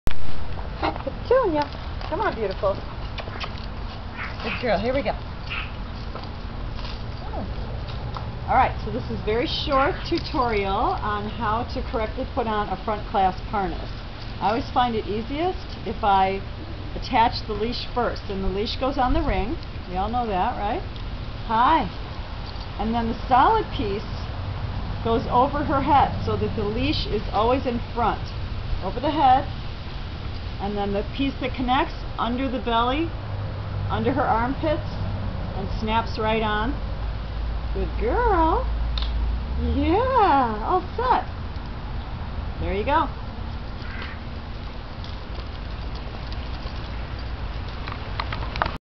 Front clasp harness attach
The correct way to attach a front clasp harness from Walk Your Dog With Love, with trainer Amy Robinson